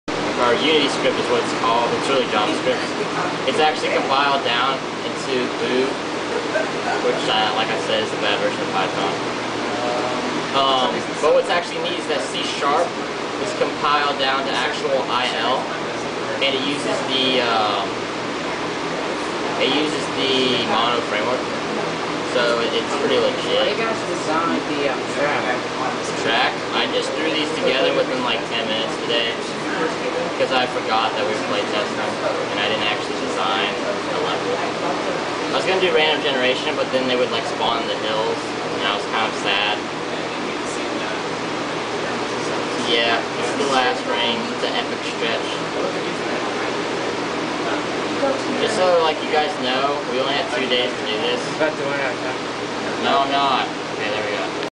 Submarine Game Developed using Unity 3-D Game Engine at Carnegie Mellon
Developed at Carnegie Mellon's Entertainment Technology Center (ETC-Global)
Game development using Unity 3-D Game Engine